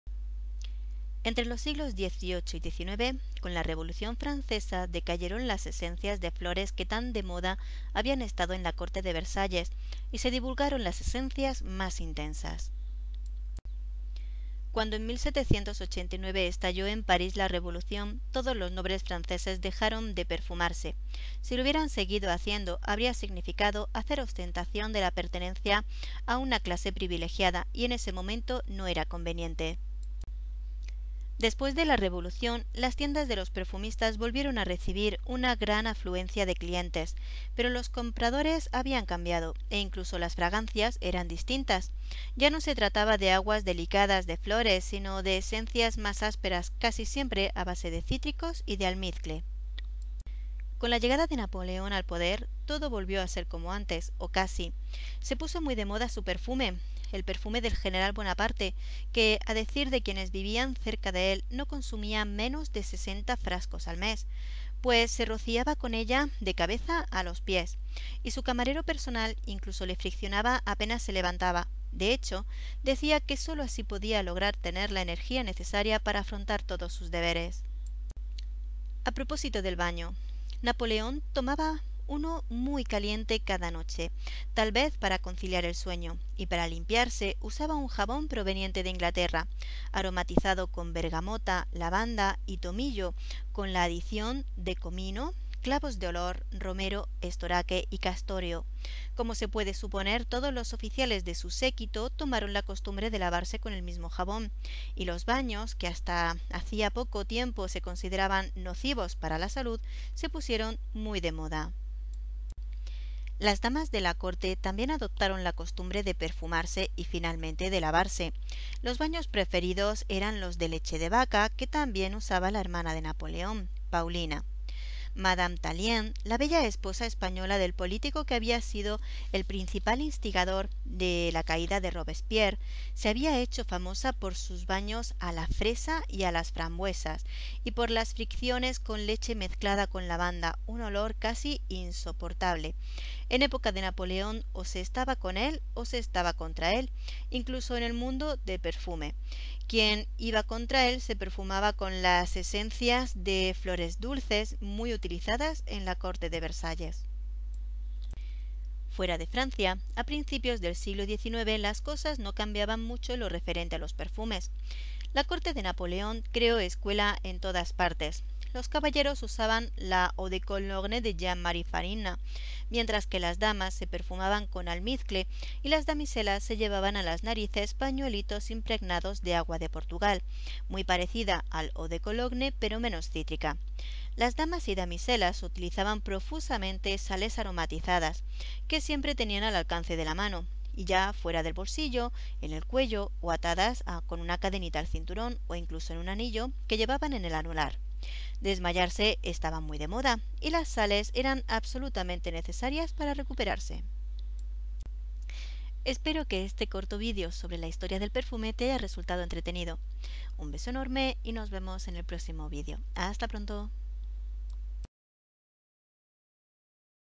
HISTORIA DEL PERFUME ♦ EL PERFUME TRAS LA REVOLUCIÓN FRANCESA parte 18
Información proveniente de la enciclopedia de El Fascinante Mundo de los Perfumes - Derechos legales cedidos por Planeta de Agostini.